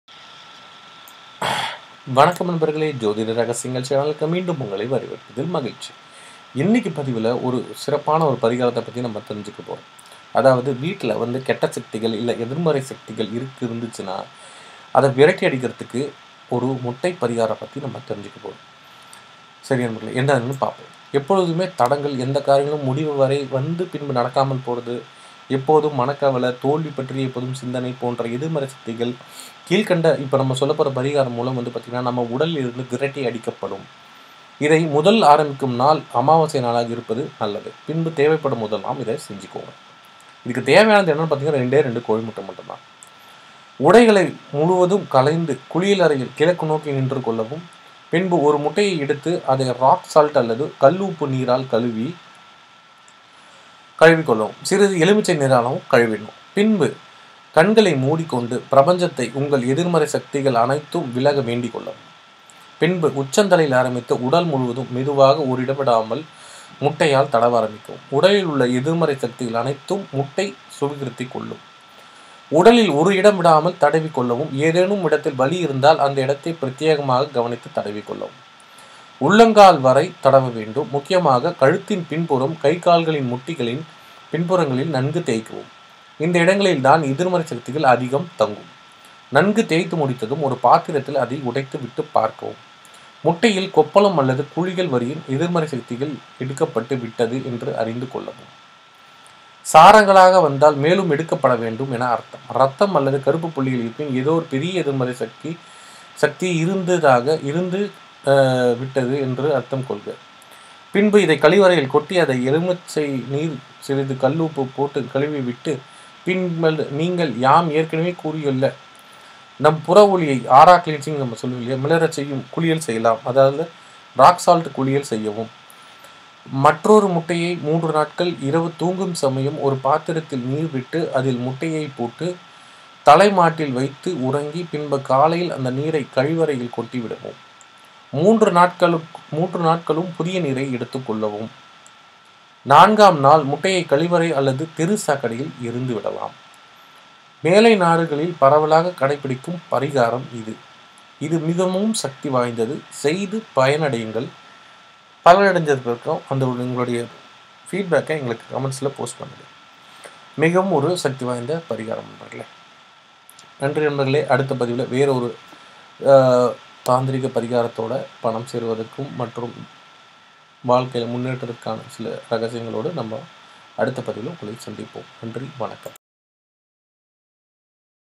வளமான வாழ்க்கைக்கு = உடலில் உள்ள தீய சக்தி விலக | எதிர்மறை சக்தி விலக முட்டை பரிகாரம்
வளமான வாழ்க்கைக்கு = உடலில் உள்ள தீய சக்தி விலக | எதிர்மறை சக்தி விலக முட்டை பரிகாரம் - வீட்டில் பண முன்னேற்றம் வந்து கொண்டே இருக்க ஒர் எளிய பரிகாரம்